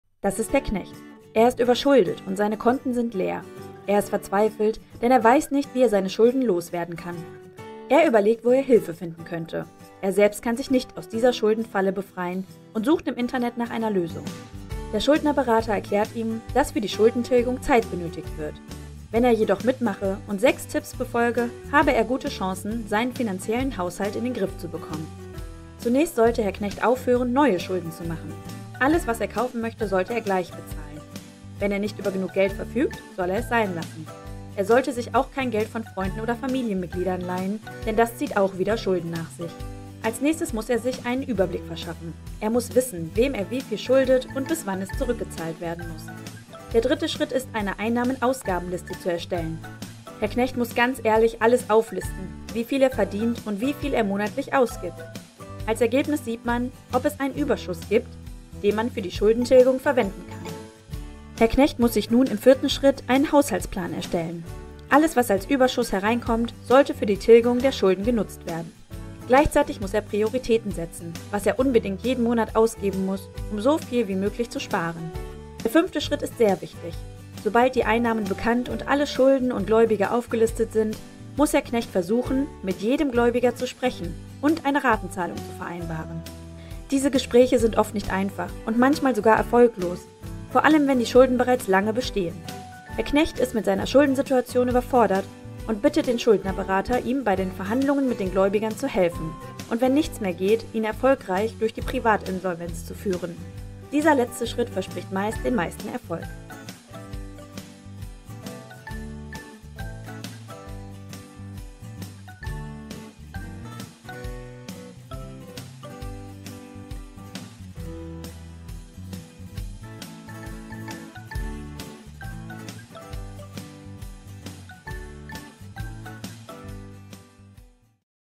Schulden loswerden: 6 Tipps zur Schuldenfreiheit
Herr Knecht ist überschuldet und seine Konten sind leer. Er ist verzweifelt, denn er weiß nicht, wie er seine Schulden loswerden kann.

Er überlegt, wo er Hilfe finden könnte. Er selbst kann sich nicht aus dieser Schuldenfalle befreien und sucht im Internet nach einer Lösung.

Zu seinem Glück stößt er auf die Schuldnerberatung Fehse.

Der Schuldnerberater erklärt ihm, dass für die Schuldentilgung Zeit benötigt wird. Wenn er jedoch mitmache und 6 Tipps befolge, habe er gute Chancen, seinen finanziellen Haushalt in den Griff zu bekommen.

Welche das sind, erfahren Sie in diesem Video.

Mehr Informationen zum Thema Schulden lsowerden finden Sie auf unserer Webseite

Schuldnerberatung Fehse
Sendlinger Straße 20
80331 München